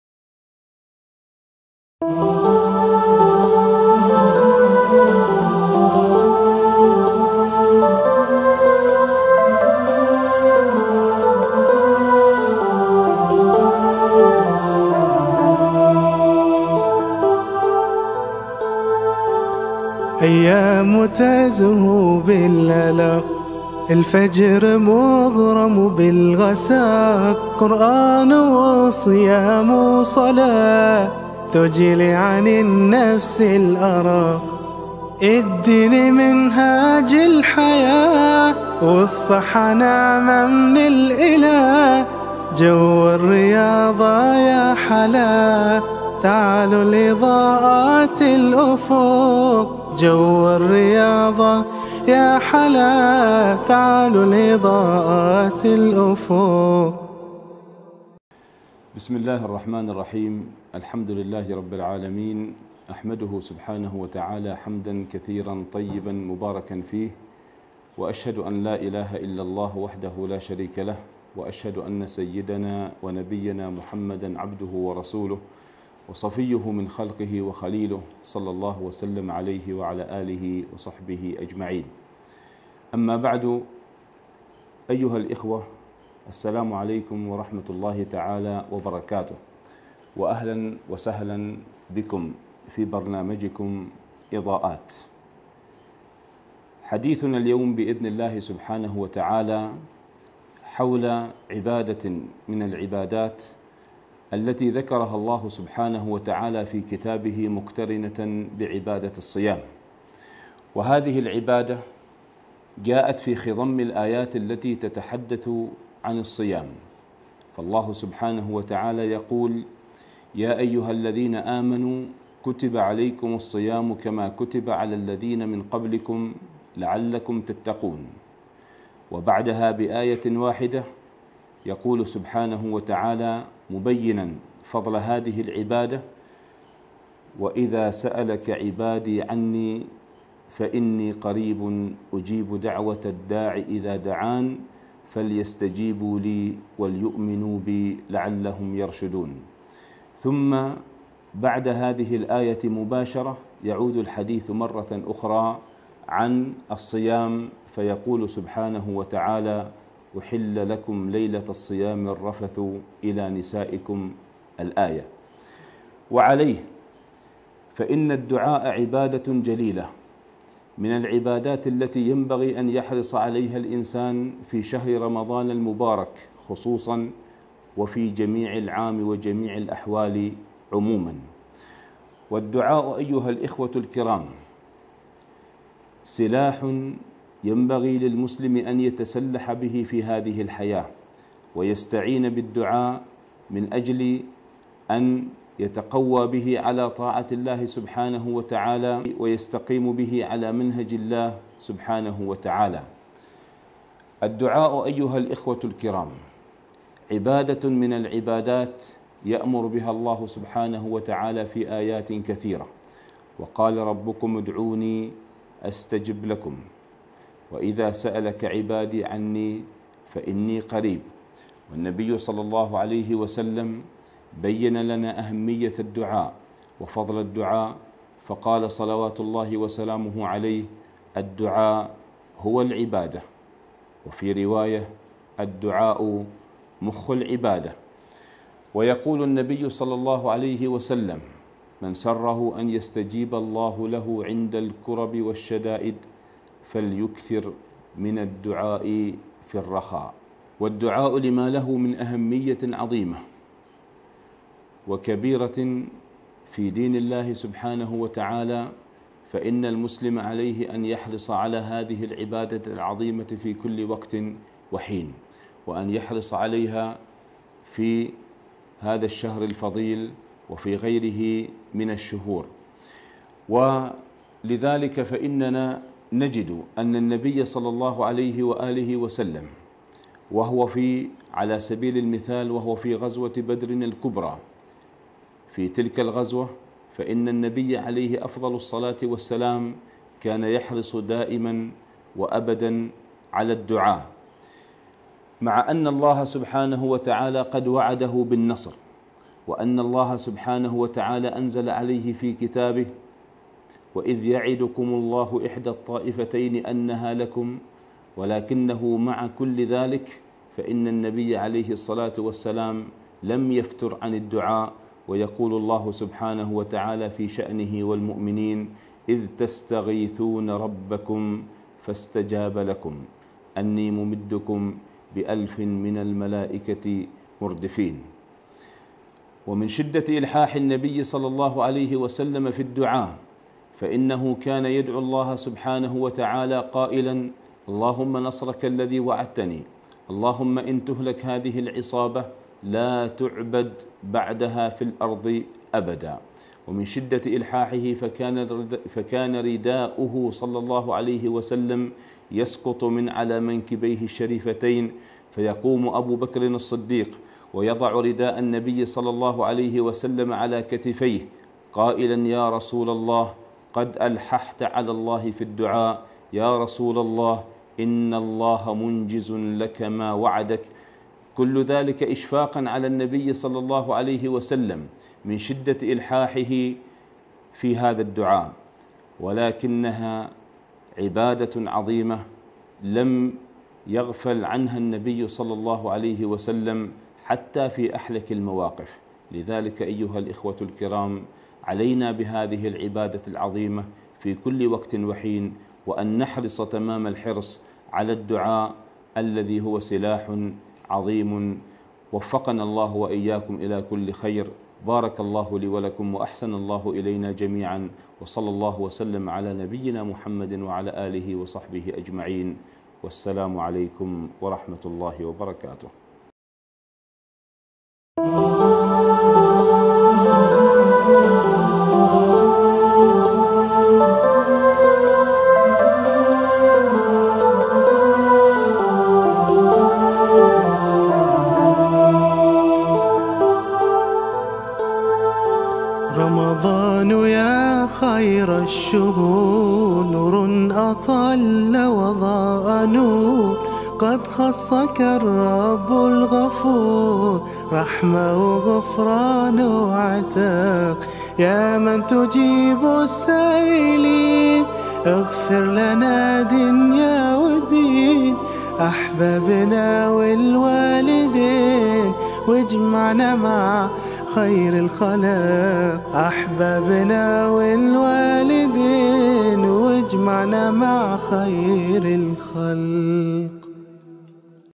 إضاءة دينية عن عبادة الدعاء وفضائلها. يُحدثنا فيها الشيخ سعيد المجرفي
مصدر المقطع:

فريق عمان لتنمية المواهب